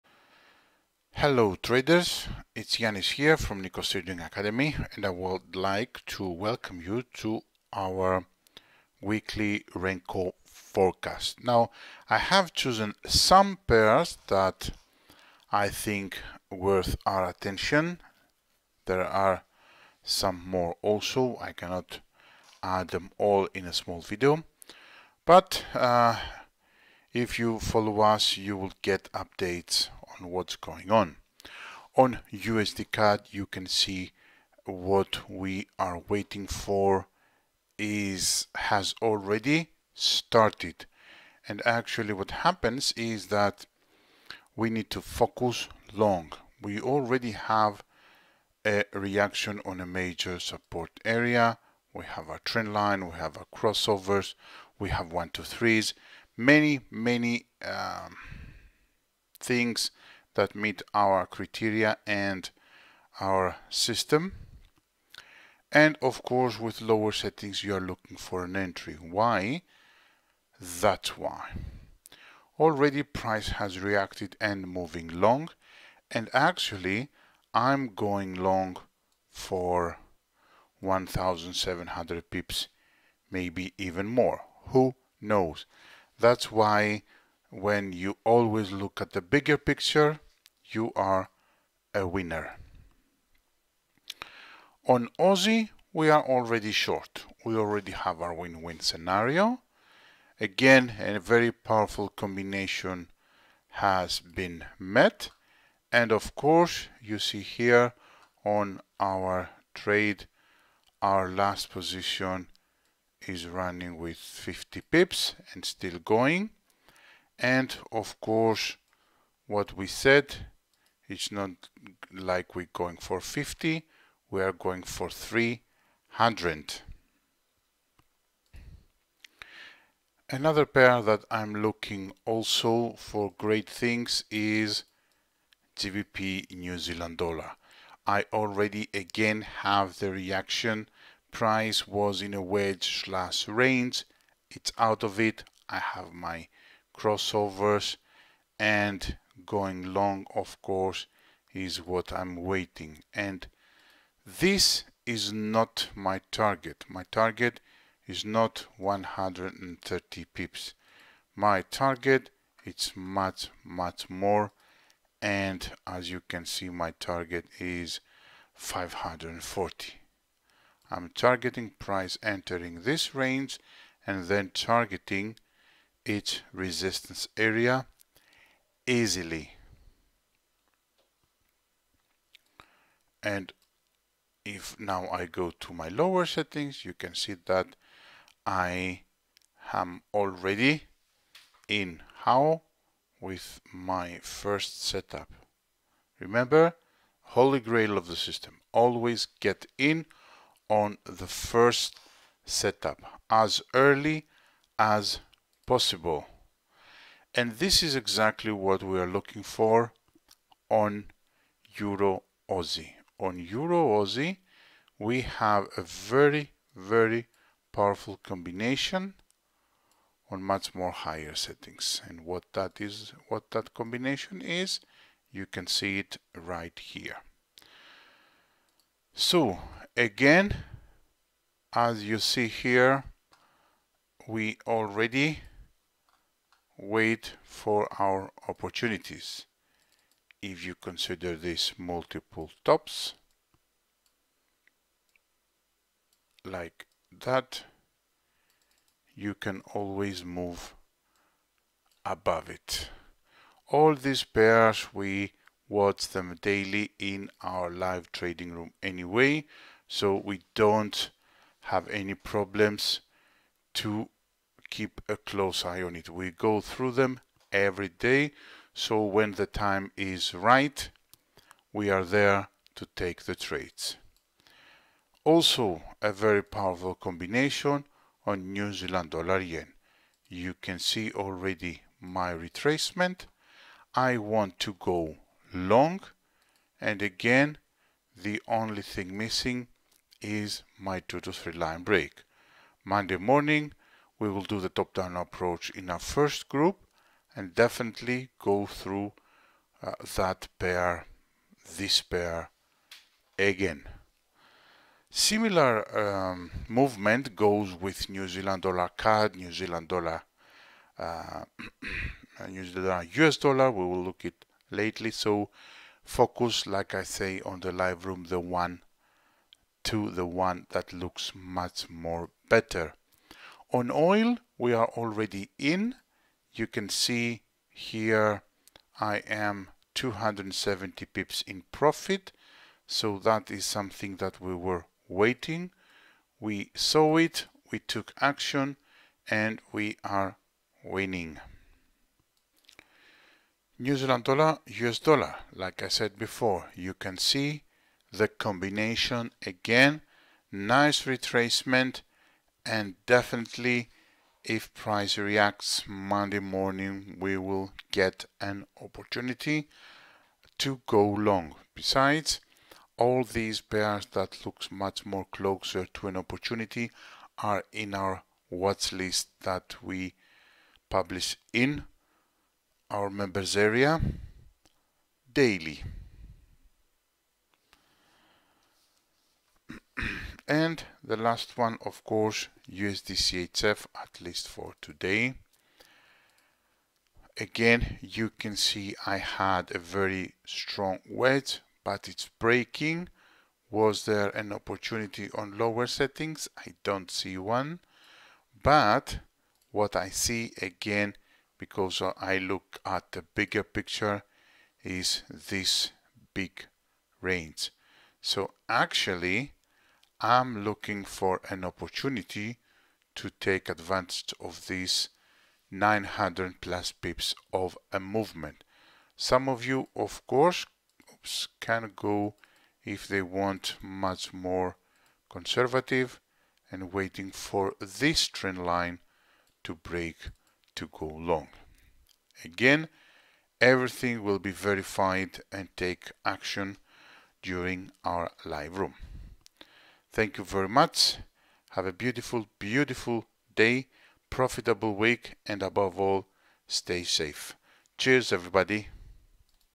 Weekly Renko Forecast 20-24 Sept 2021
00:00 - USDCAD
01:34 - AUDUSD
02:13 - GBPNZD
03:30 - EURAUD
04:35 - NZDJPY
05:42 - NZDUSD
06:25 - USDCHF

Enjoy the Weekly Renko Forecast and remember to comment, share and like this video!

Happy & Healthy Trading
Nikos Trading Academy Team